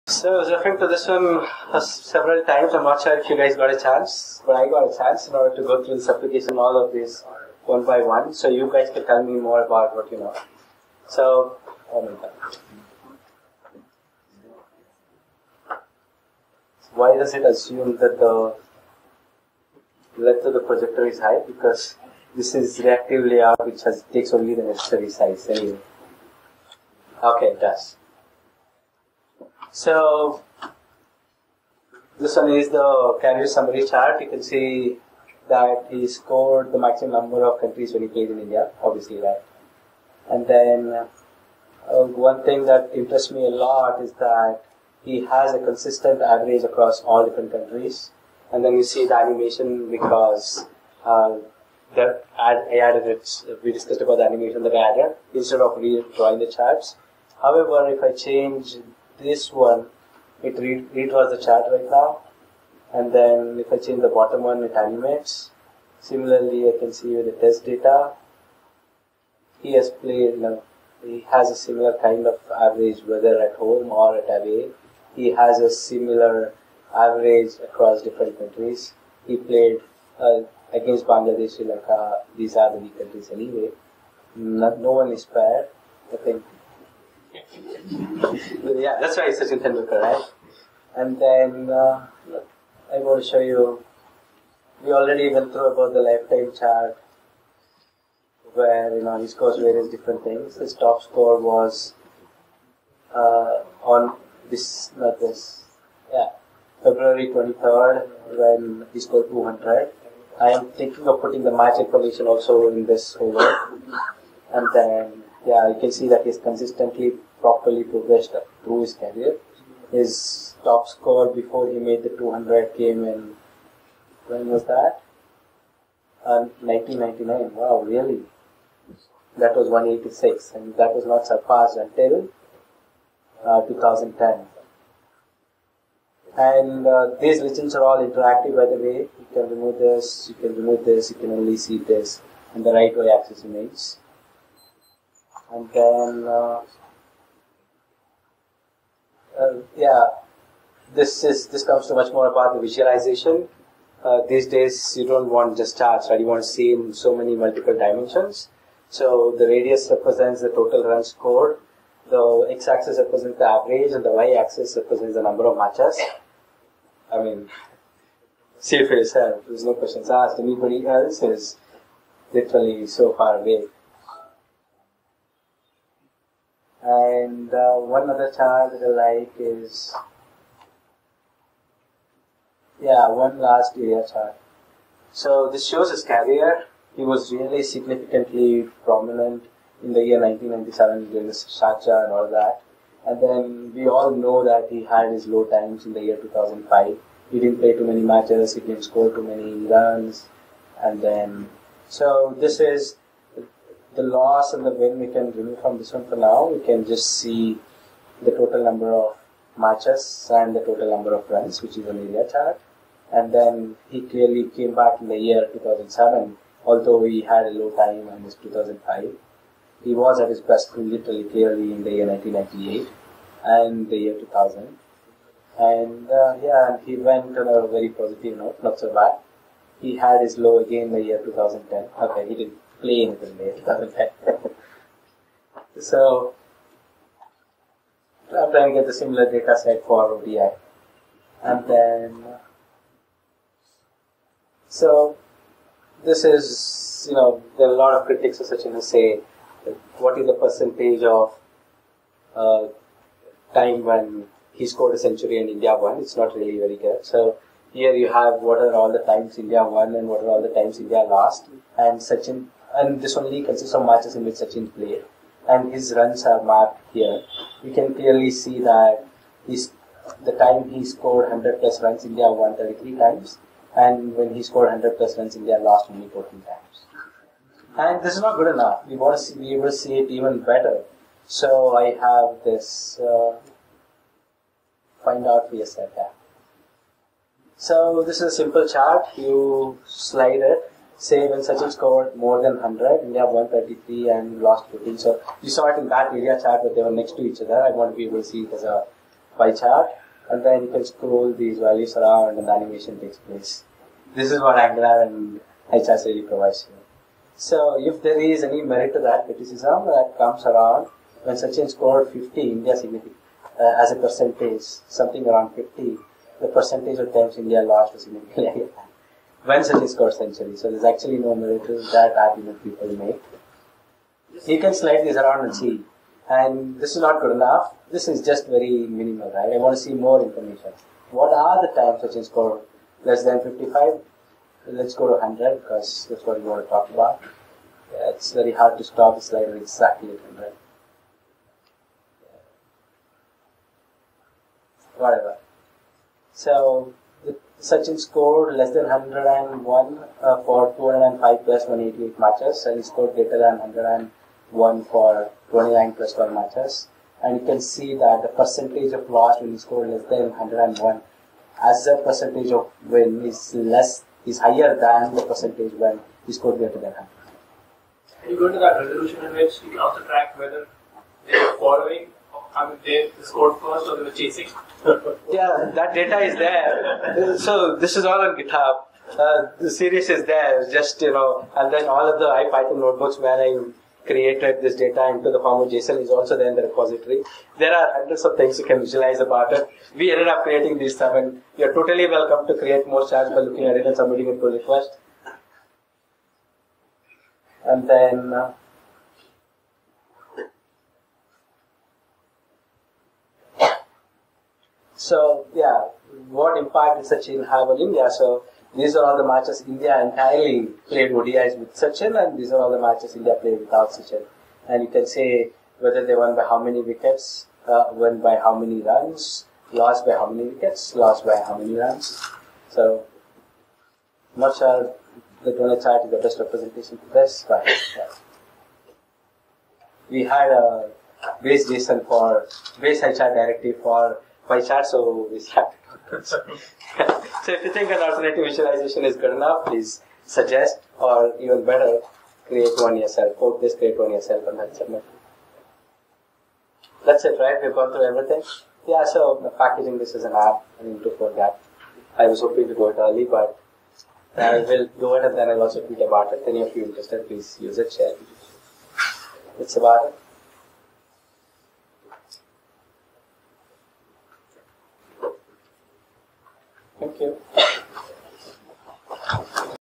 Building Highly Interactive Charts using Angular Directives
Lakshman Prasad continues his earlier talk by showing off the charts he's built with AngularJS and Highcharts, at the AngularJS Miniconf by HasGeek on Feb 11, 2014.

This video has somewhat muffled audio because we had to apply heavy noise cancellation to remove background noise.